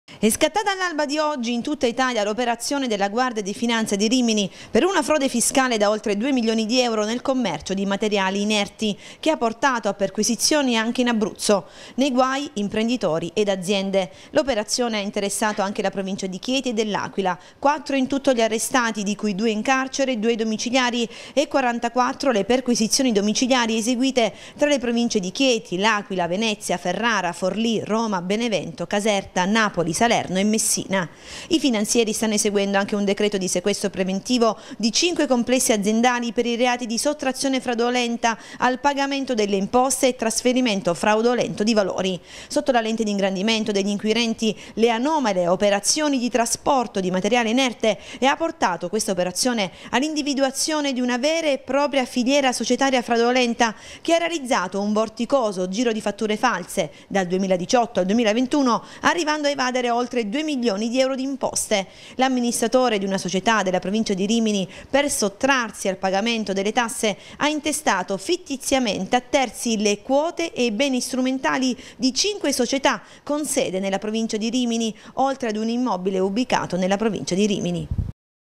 Cronaca | Maxi evasione fiscale: la Finanza effettua 4 arresti tra L'Aquila e Chieti
| SUPER J |

Contatti Redazione:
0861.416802